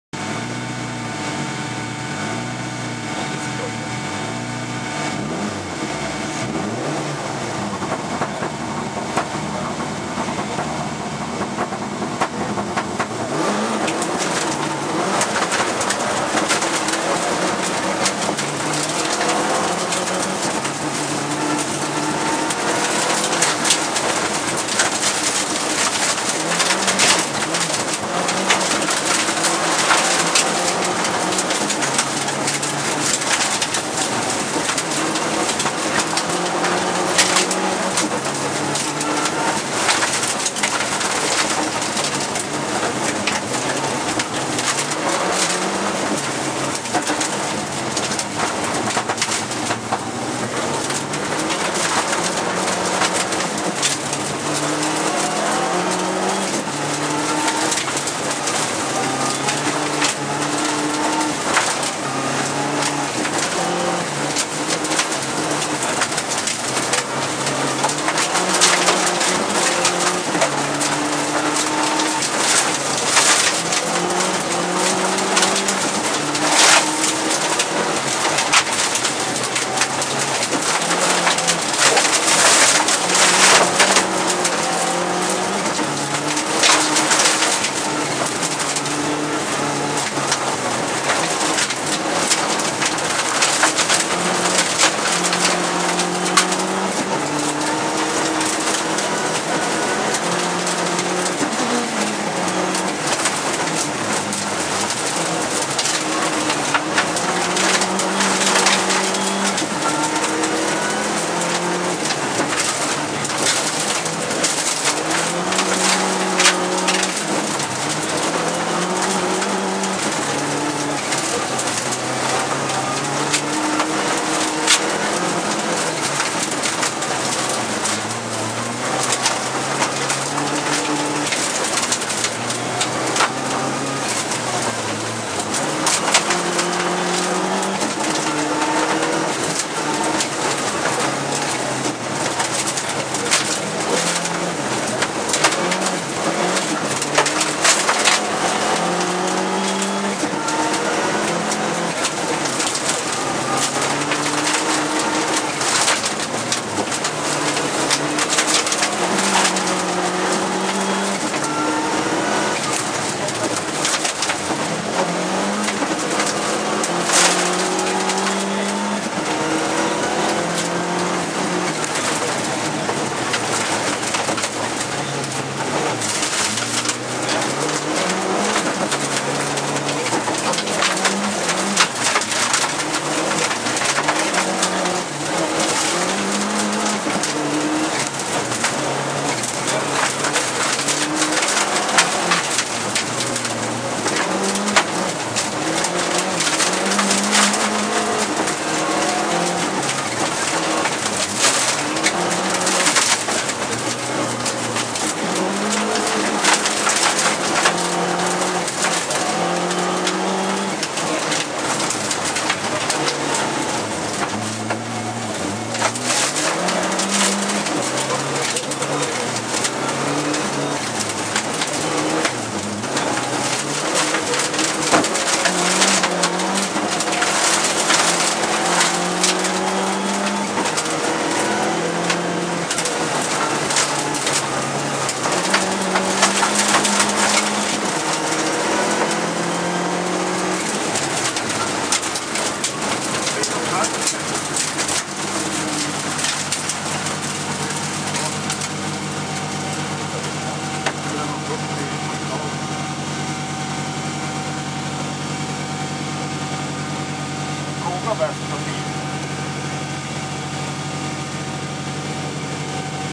WRC Onboard 2010 EMK kannan.SS2. Lancia Integrale 16v . Jan-olof Marklund
SS2  Problem with respons system and only 1,1 bar Turbo presure, god time time after all.